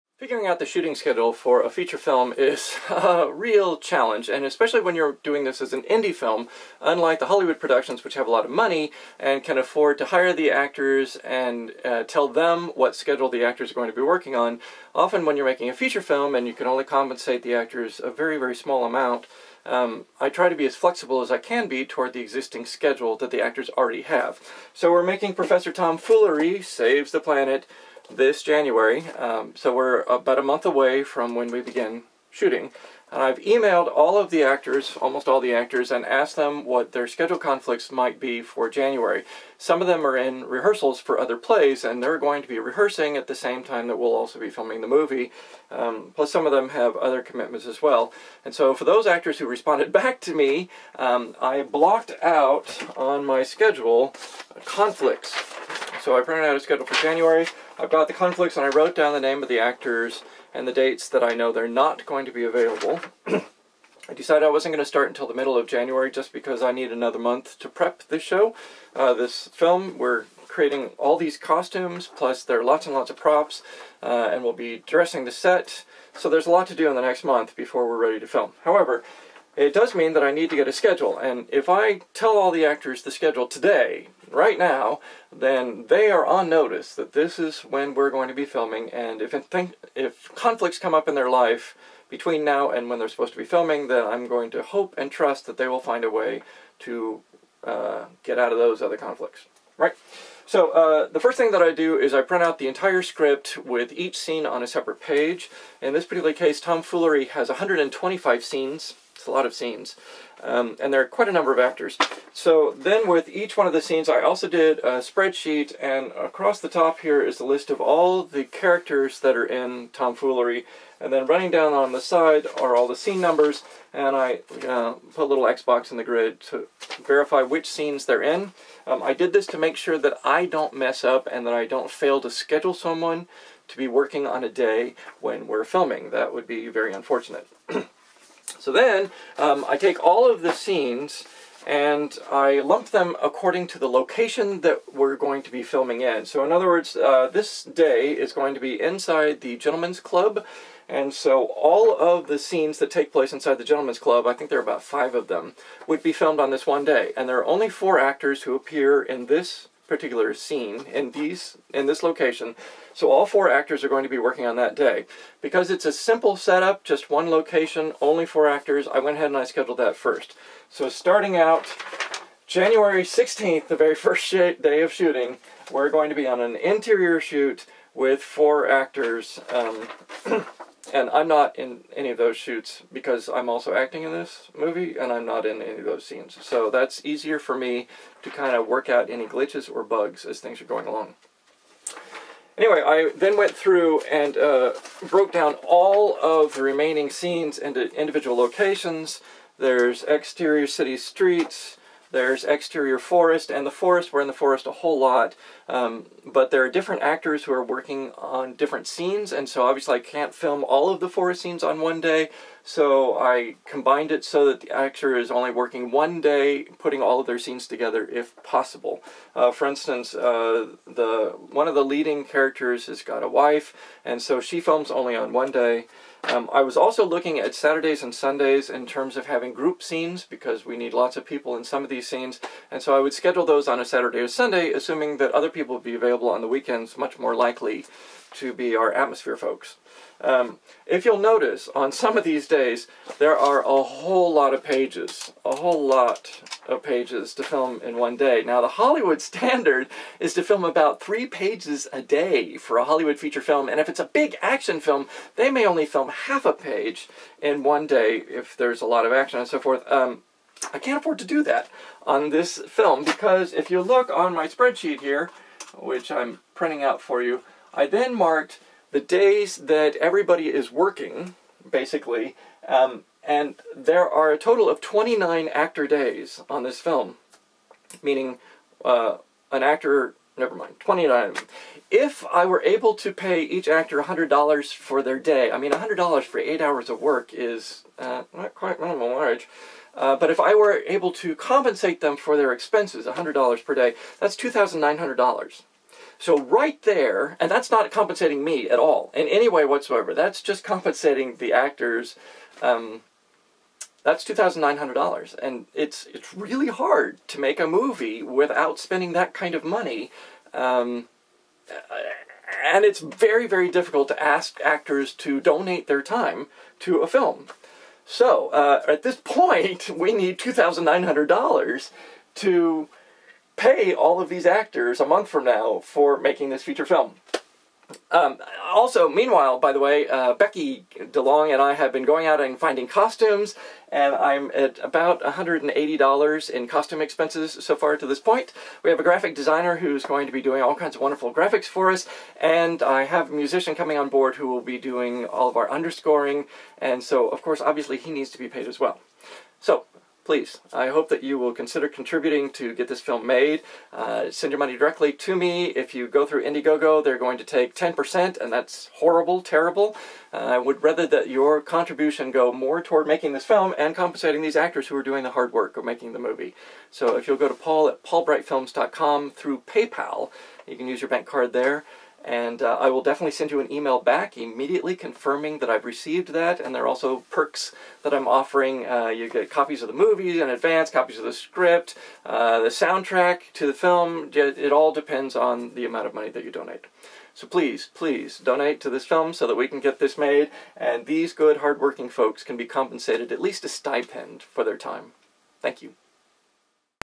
How to Schedule Film Shoot
Scheduling an indie film requires a LOT to work around actors' schedules, location availability, keeping the budget as lean as possible AND having back up plans in the event of snowmageddon. This is how I did it for PROF TOM FOOLERY SAVES THE PLANET. With Alex McCracken, Jonas D. Israel, Joe Cronin, Walter Petryk, Phillip A. Rudolph, Michael Salzburg-Felts, Kyle Kinglish, Chris Patterson, Rebecca Ross DeLong and Torrey Newhart.